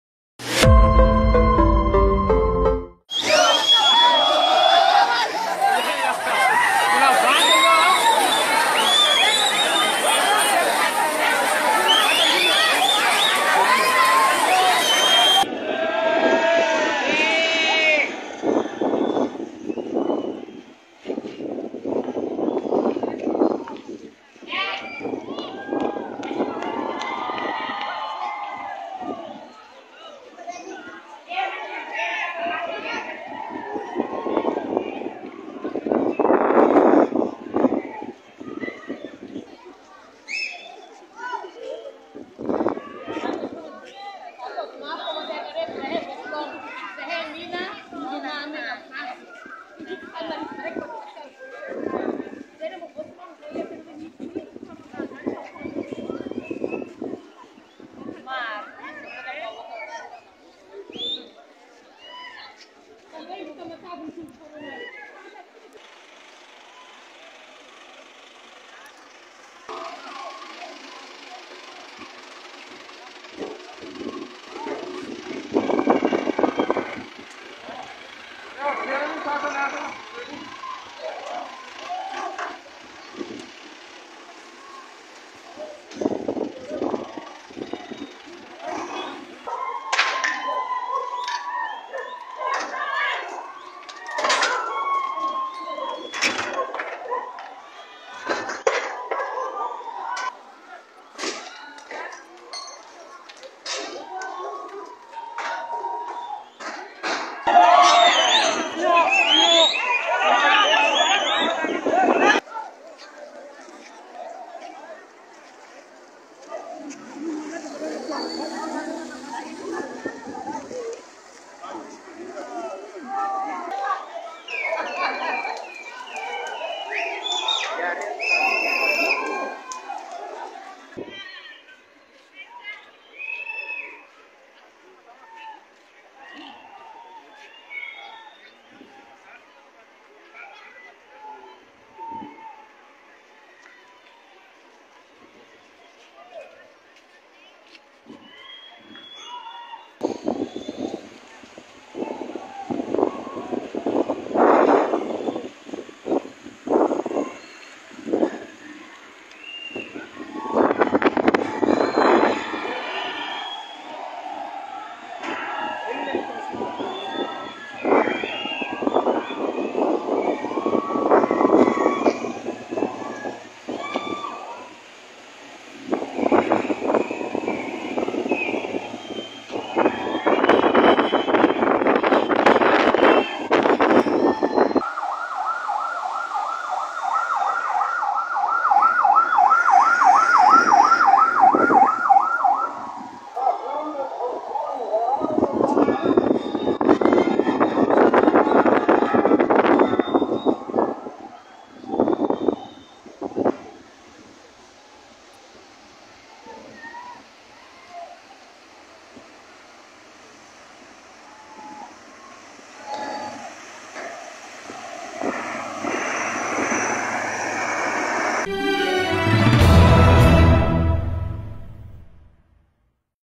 TOAMASINA – Raikitra ny tora bato, nirefotra ny gazy mandatsa dranomaso tao Androranga
Nifandona tao Androranga-Toamasina ny mpitandro ny filaminana sy ireo mponina manodidina io alarobia tolakandro io. Naneho tsy fanekenny ny fihetsiky ny mpitandro ny filaminana tamina lehilahy iray teny an-dalana ireto mponina ka raikitra ny tora-bato. Namaly tamin'ny gazy mandatsa-dranomaso sy tifi-danitra ny mpitandro ny filaminana.